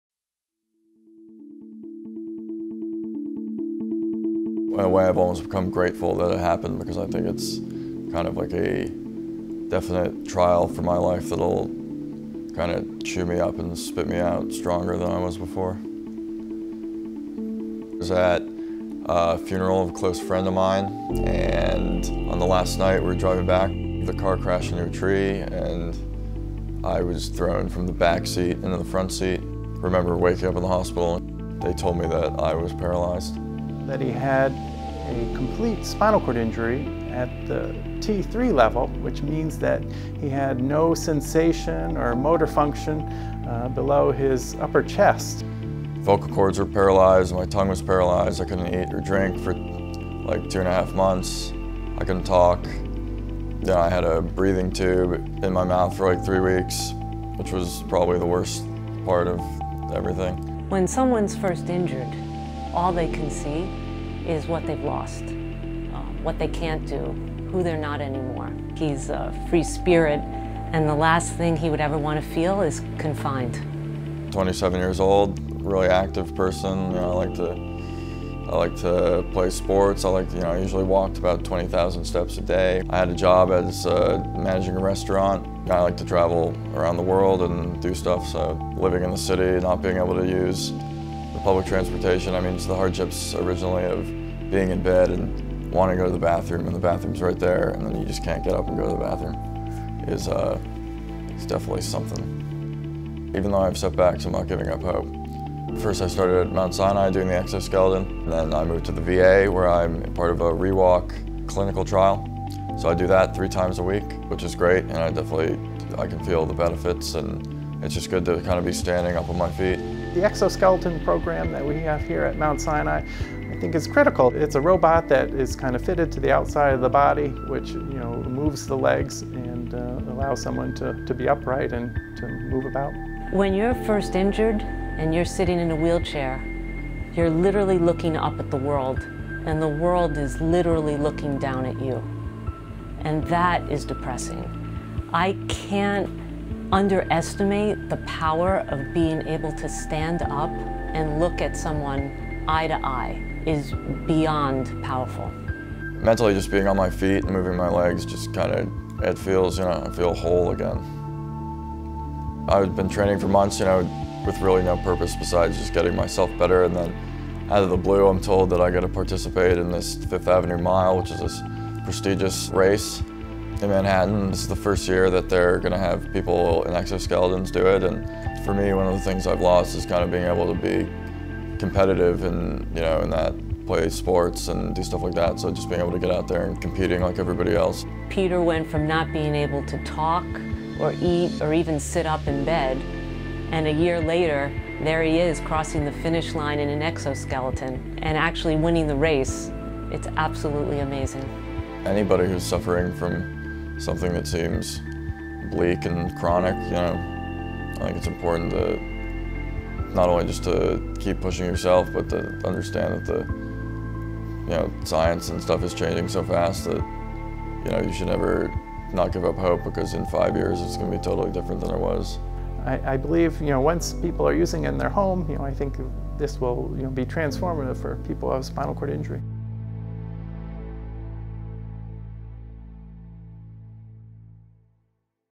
SCI Patient Races in 5th Avenue Mile Using Exoskeleton
The sixth episode of Mount Sinai Future You highlights inspirational stories and features a young man’s journey following a horrific car accident that left him paralyzed. With the help of the exoskeleton program, Thomas Bryce, MD, Director of the Spinal Cord Injury Program at Mount Sinai, and Angela Riccobono, PhD, Senior Clinical Psychologist of Rehabilitation Medicine at Mount Sinai, he was able to participate in the 2018 New Balance 5th Avenue Mile Race sponsored by the New York Road Runners.